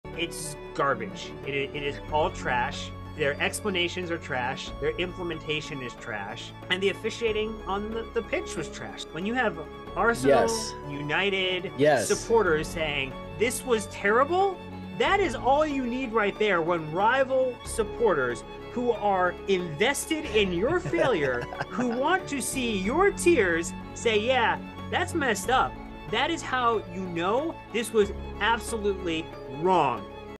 VAR IS BROKEN!
VAR costing Chelsea 3 points once again. How should the PGMOL fix the broken system?